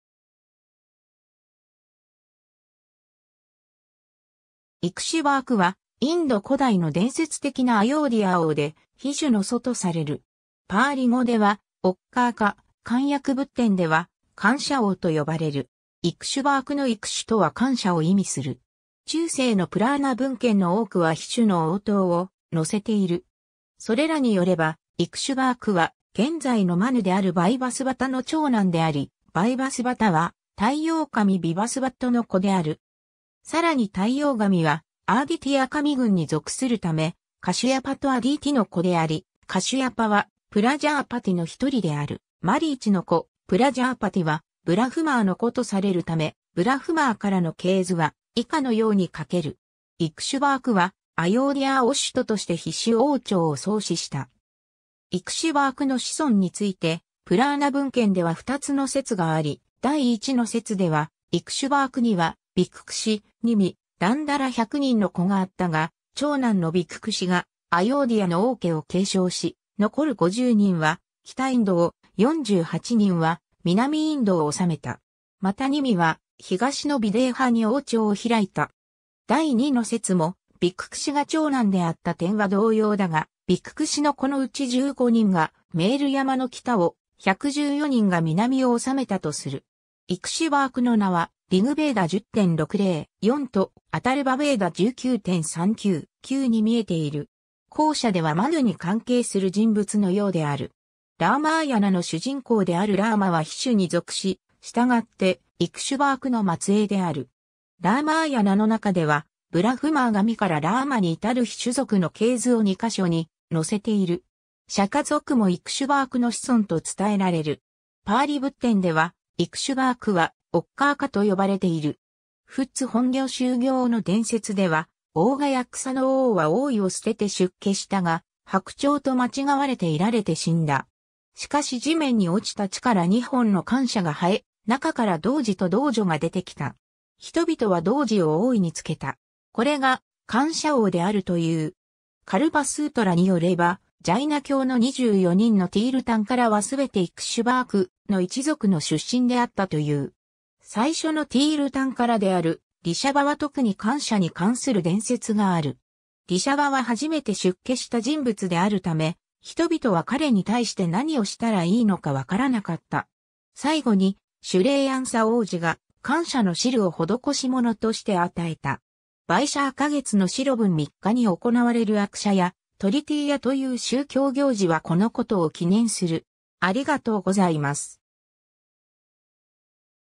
イクシュヴァーク
イクシュヴァーク（サンスクリット語: इक्ष्वाकु Ikṣvāku）は、インド古代の伝説的なアヨーディヤー王で、日種の祖とされる。
パーリ語ではオッカーカ（Okkāka）、漢訳仏典では甘蔗王（かんしょおう）と呼ばれる。
 イクシュヴァークのイクシュ（ikṣu）とは甘蔗（サトウキビ）を意味する。
 中世のプラーナ文献の多くは日種の王統を載せている。
それらによれば、イクシュヴァークは現在のマヌであるヴァイヴァスヴァタの長男であり、ヴァイヴァスヴァタは太陽神ヴィヴァスヴァットの子である。
さらに太陽神はアーディティヤ神群に属するためカシュヤパとアディティの子であり、カシュヤパはプラジャーパティのひとりであるマリーチの子、プラジャーパティはブラフマーの子とされるため、ブラフマーからの系図は以下のように書ける。
 イクシュヴァークはアヨーディヤーを首都として日種王朝を創始した。
イクシュヴァークの子孫についてプラーナ文献では2つの説があり、第1の説ではイクシュヴァークにはヴィククシ（シャシャーダとも）、ニミ（ネーミ）、ダンダ（ダンダカ）ら100人の子があったが、長男のヴィククシがアヨーディヤの王家を継承し、残る50人は北インドを、48人は南インドを治めた。
またニミは東のヴィデーハに王朝を開いた。
第2の説もヴィククシが長男であった点は同様だが、ヴィククシの子のうち15人がメール山の北を、114人が南を治めたとする。
 イクシュヴァークの名は『リグ・ヴェーダ』10.60.4と『アタルヴァ・ヴェーダ』19.39.9に見えている。
後者ではマヌに関係する人物のようである。
 『ラーマーヤナ』の主人公であるラーマは日種に属し、したがってイクシュヴァークの末裔である。
『ラーマーヤナ』の中ではブラフマー神からラーマにいたる日種族の系図を2か所に載せている(1.70, 2.110)。
 釈迦族もイクシュヴァークの子孫と伝えられる。
パーリ仏典ではイクシュヴァークはオッカーカと呼ばれている。
『仏本行集経』の伝説では、大茅草王は王位を捨てて出家したが、白鳥とまちがわれて射られて死んだ。
しかし地面に落ちた血から2本の甘蔗が生え、中から童子と童女が出てきた。
人々は童子を王位につけた。
これが甘蔗王であるという。
 『カルパ・スートラ』によればジャイナ教の24人のティールタンカラはすべてイクシュヴァークの一族の出身であったという。
最初のティールタンカラであるリシャバはとくに甘蔗に関する伝説がある。
リシャバは初めて出家した人物であるため、人々は彼に対して何をしたらいいのかわからなかった。
最後にシュレーヤンサ王子が甘蔗の汁を施し物として与えた。
ヴァイシャーカ月の白分3日に行われるアクシャヤ・トリティーヤ（英語版）という宗教行事はこのことを記念する。